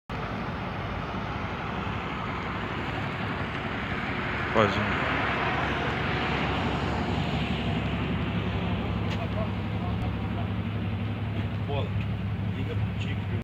PRF apreende 250 mil carteiras de cigarro contrabandeado na BR 376
Imagens: PRF